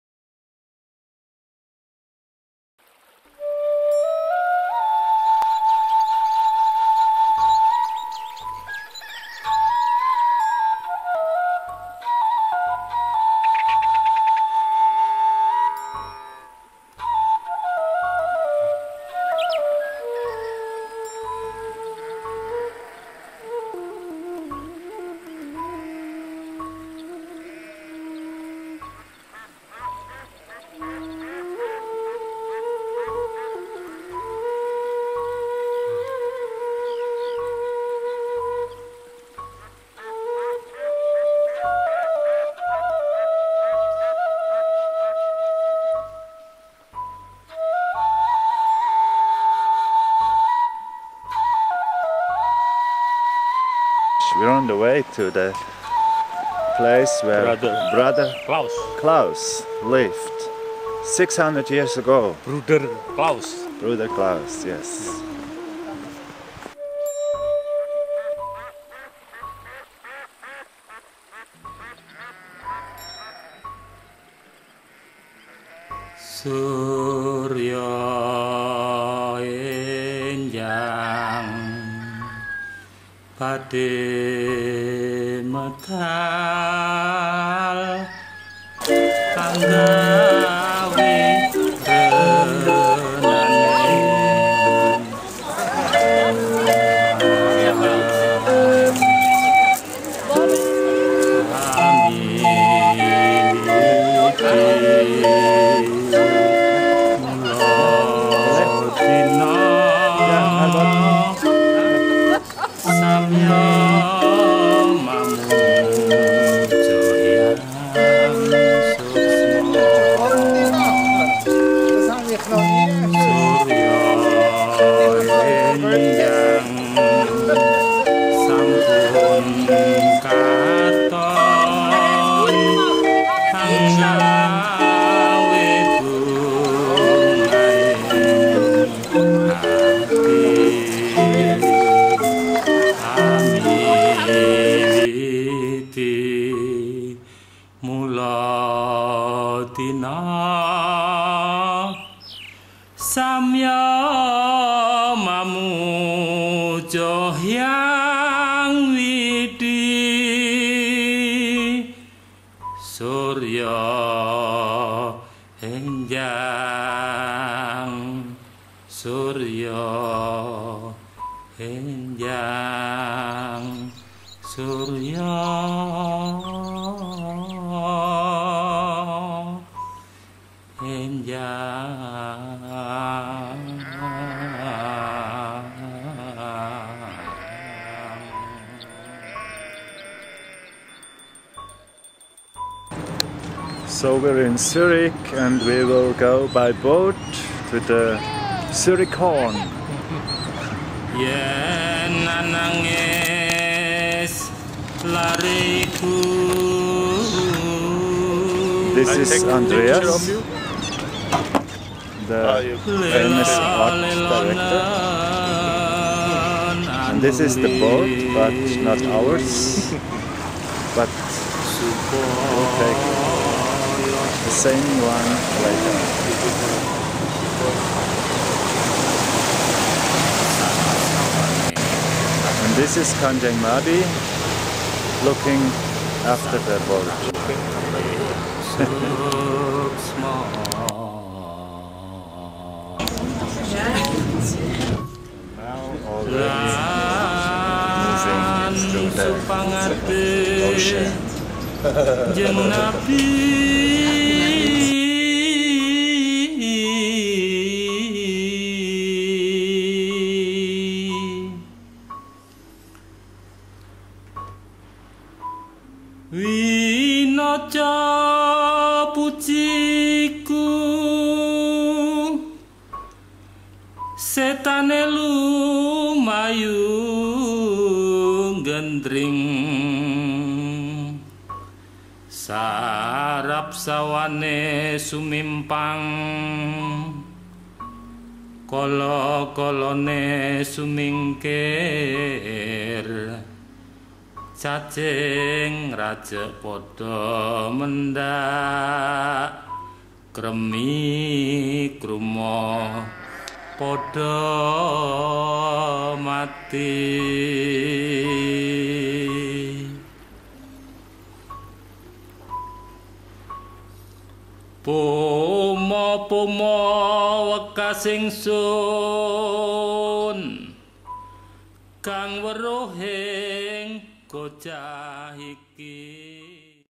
Daya Putih: Kanjeng Madi in Switzerland 2011
Every year Kanjeng Madi, from Indonesia, travels to Switzerland, to teach his students and to travel in Europe. This year he held seminars at Kurszentrum Winkel, Märstetten and at Kurszentrum Waldhaus, Lützelflüh. Later, he went to see "Mystik" at Museum Rietberg, he visited the place of the famous saint Niklaus von Flüh ("Bruder Klaus") and he spent an afternoon at Schloss Hallwyl. Of course, he also visited some friends...  (by Jack)